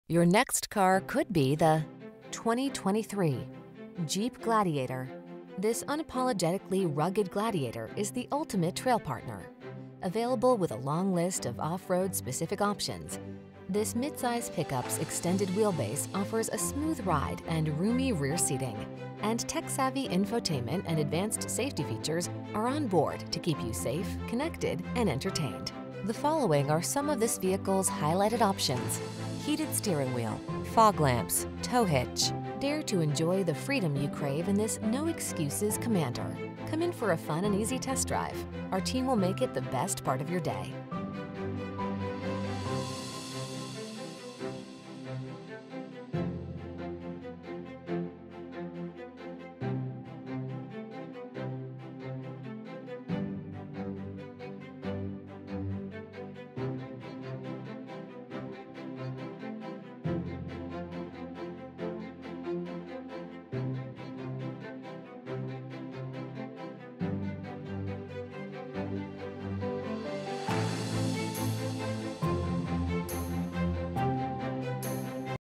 2023 Jeep Gladiator Rubicon 4x4 TN Knoxville, Farragut, Oak Ridge, Maryville, Gatlinburg
New 2023 Jeep Gladiator Rubicon 4x4 available at West Knoxville CDJR located in 8544 Kingston Pike, Knoxville, TN, 37919. Servicing the Knoxville, Farragut, Oak Ridge, Maryville, Gatlinburg area.
Please mention the stock PL571425 when you contact the dealership for this vehicle.